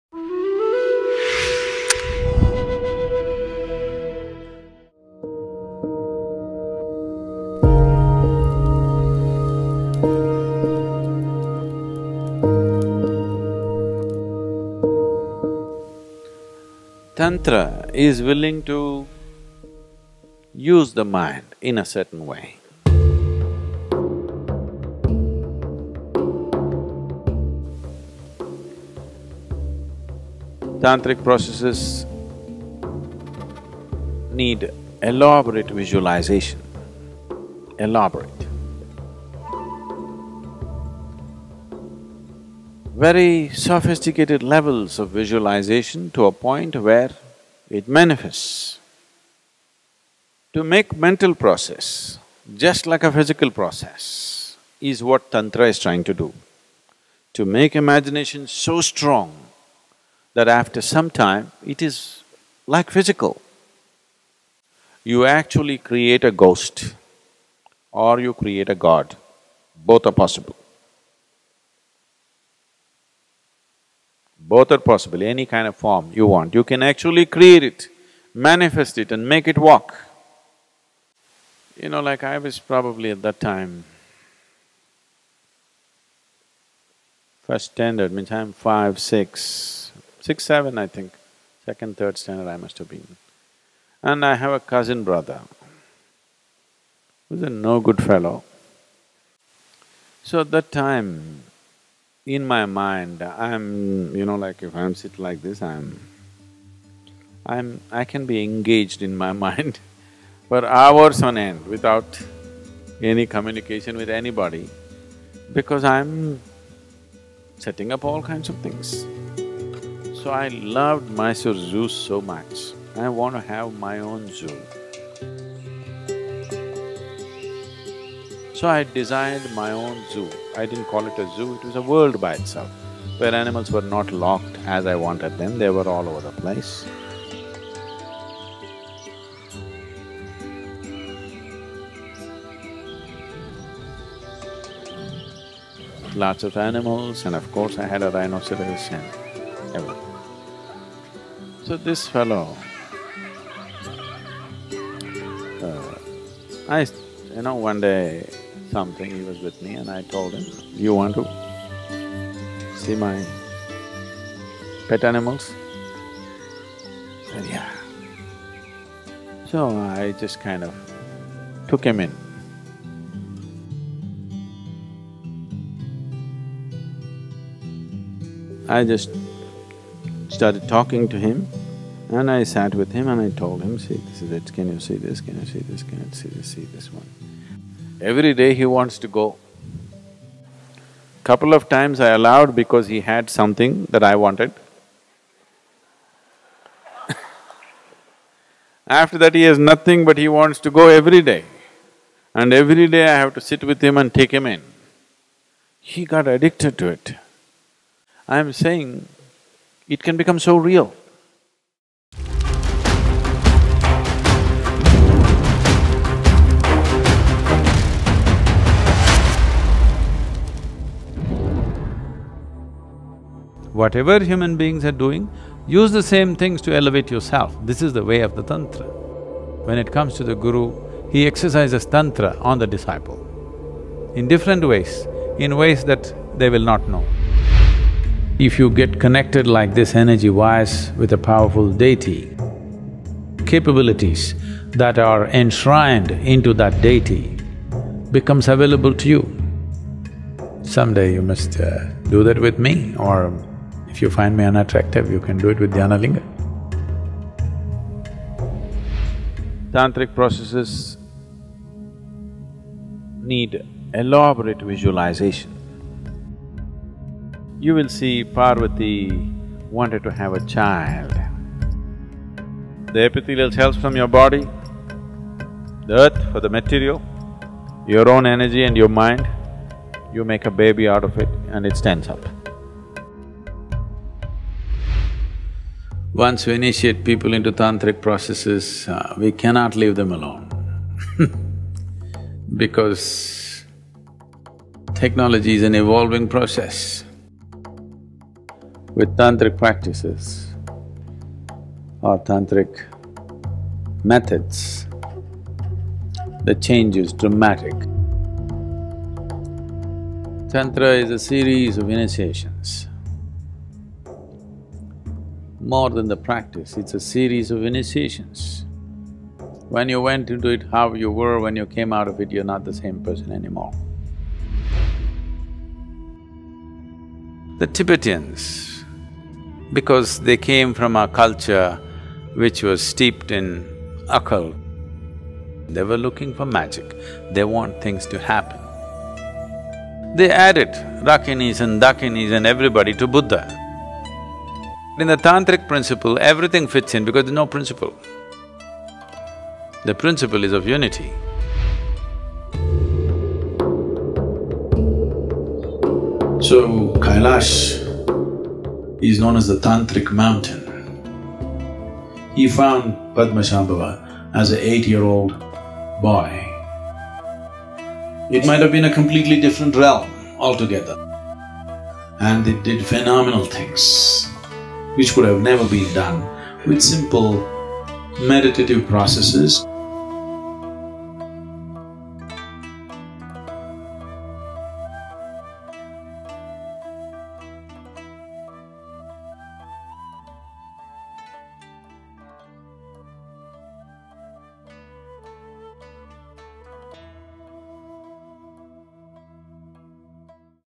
The Unbelievable Power of Imagination - Sadhguru Exclusive
Sadhguru explains how Tantra uses the mind in such a way that the imagination manifests as reality. He recalls how as a child, he created a zoo of animals with his mind, and took his cousin into it, with unexpected consequences!